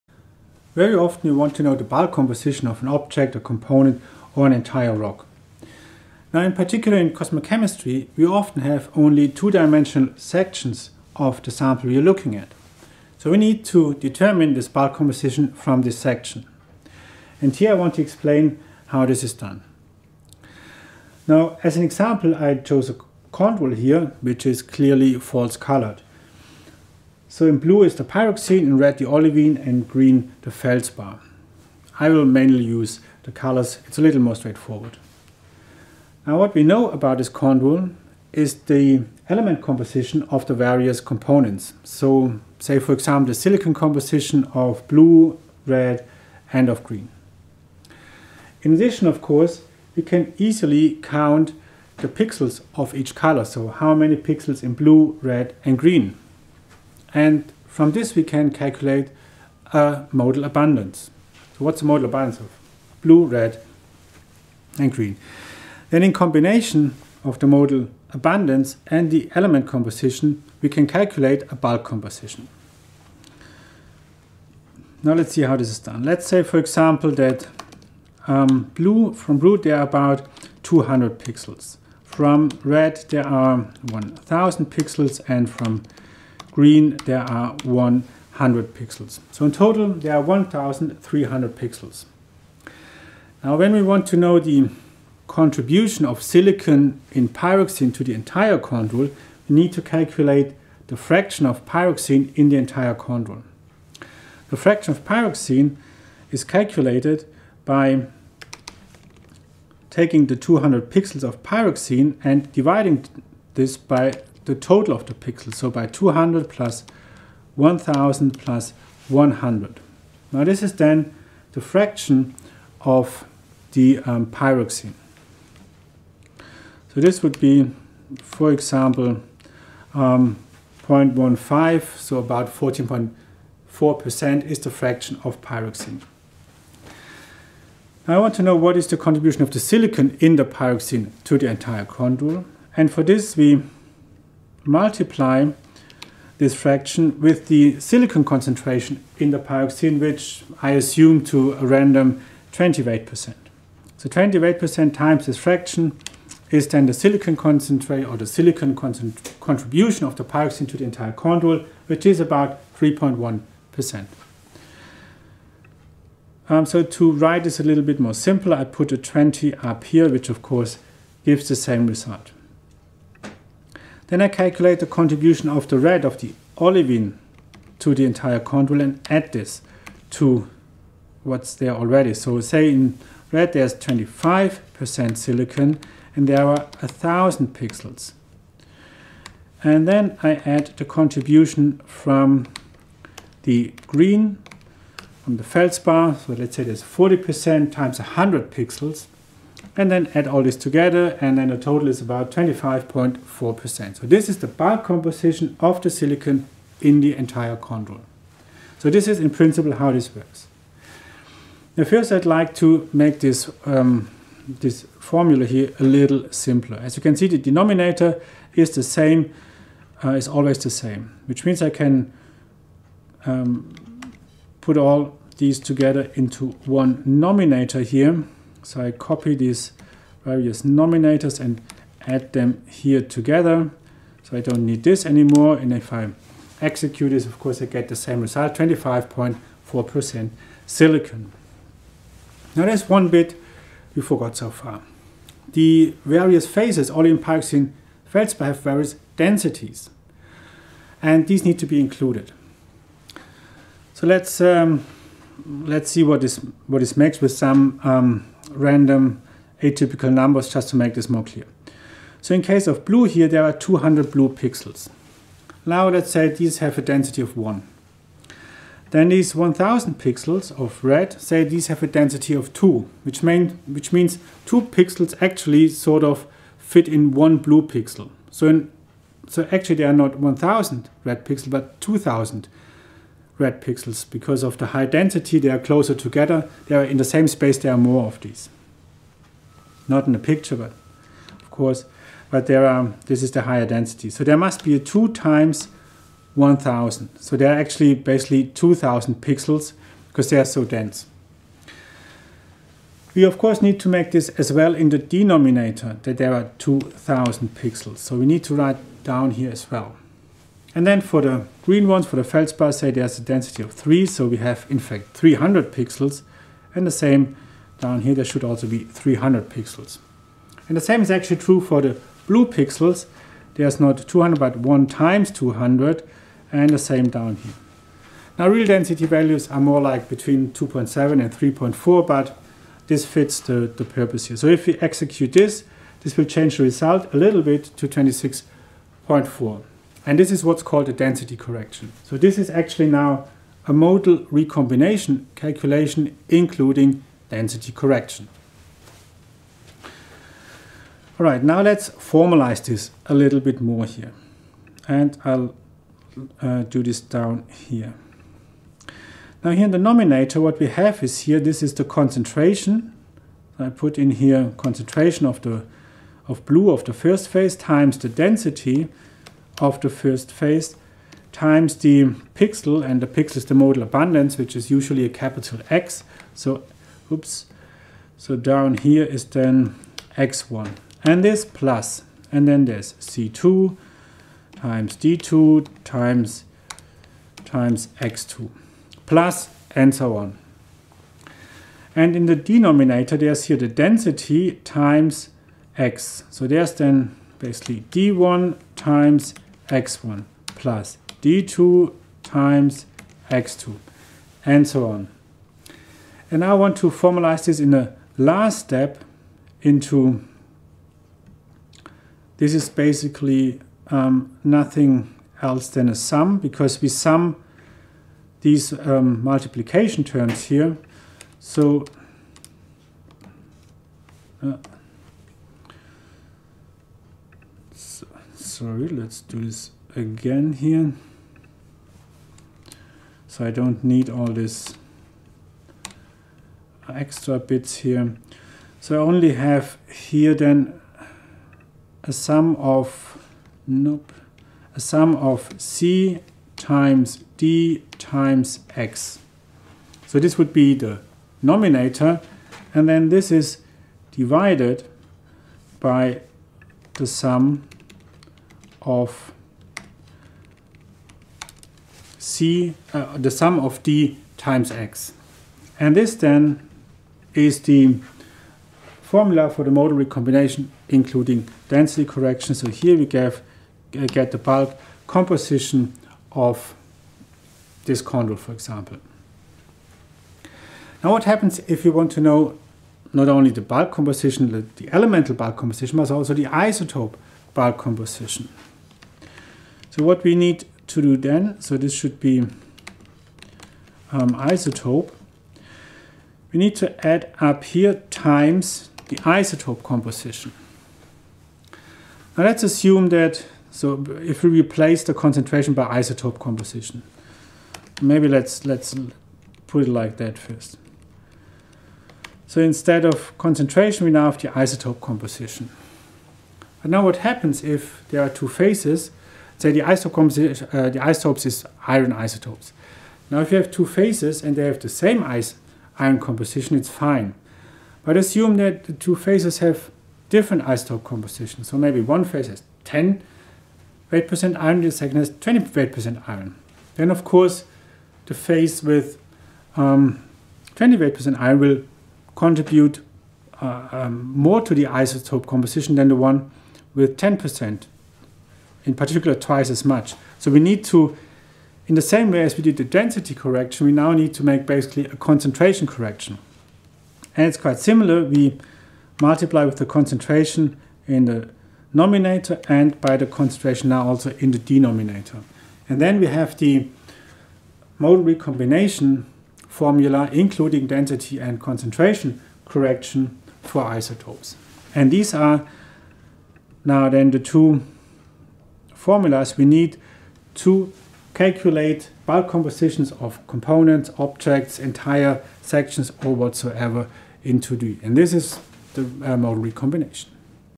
Modal Recombination with Density Correction
The bulk element or isotope composition of a meteorite section or individual components in a section such as chondrules or CAIs can be calculated from the abundances and compositions of the individual phases – given these are known –, using modal recombination. It is then required to also correct for the various densities of the individual phases. This calculation is also used for mass-balances.